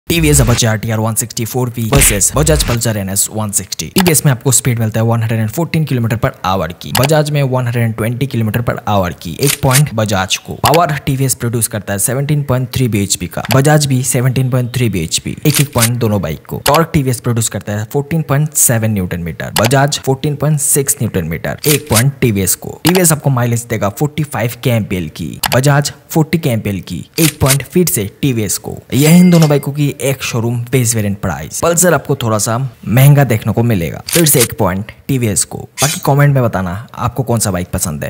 Apache RTR 160 4v VS Pulsar NS 160
Comparison of TVS apache RTR 160 4v and Bajaj Pulsar NS 160.